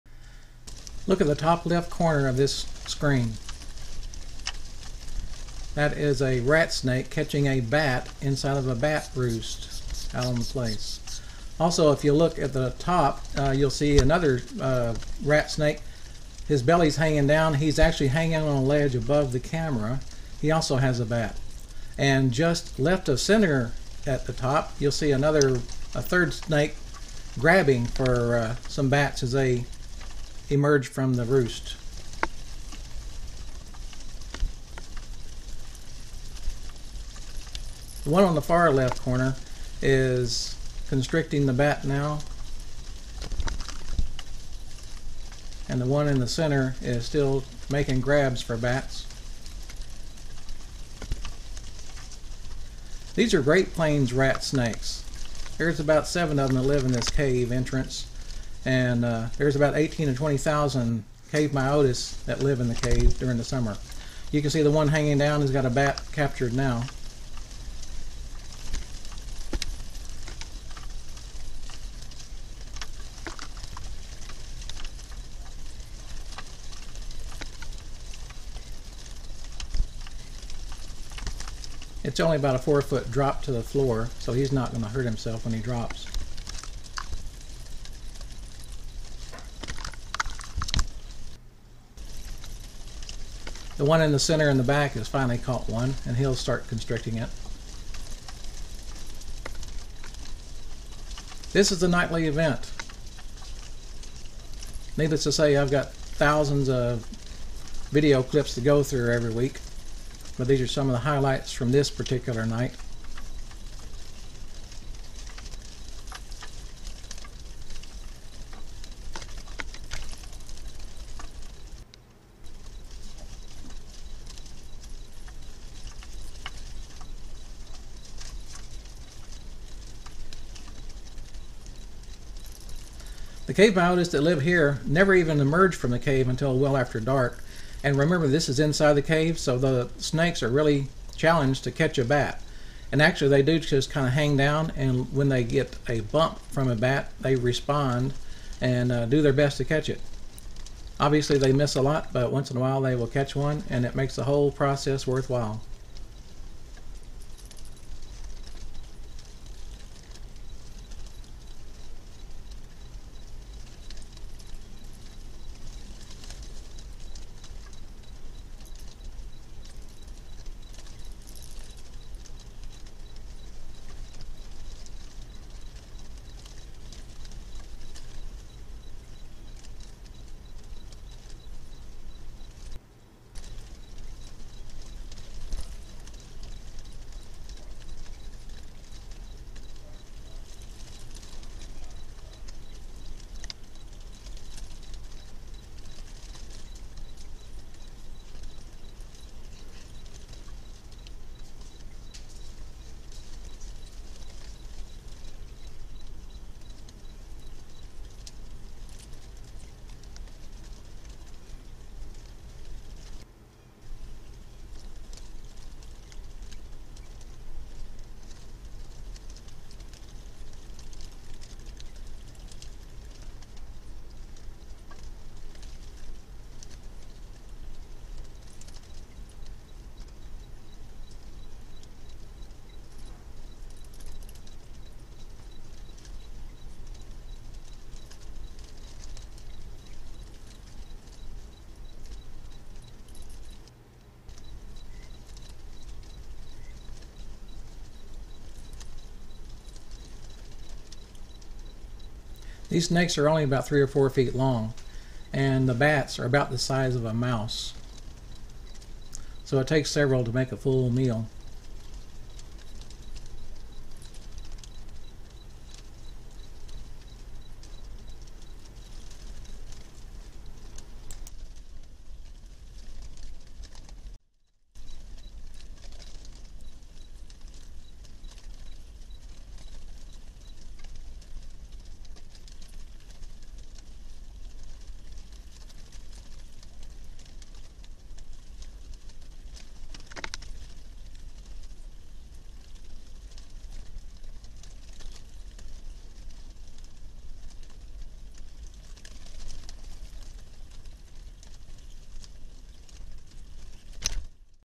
Snakes catch bats in cave
This video gives you the chance to watch Great Plains Rat Snakes catch and eat Cave Myotis at a cave roost in central Texas.  This cave has a summer population of approx. 18-20 thousand cave myotis every year.  Needless to say, that concentrated number of prey items will attract predators and the most common predator at this site is the Great Plains Rat Snake.  Other predators visit the cave but these snakes live here.   Watch how the snakes hang off the cave ledges, capture, and consume their prey.  Nature isn't always pretty (esp. if you're a bat) but that's they way it works.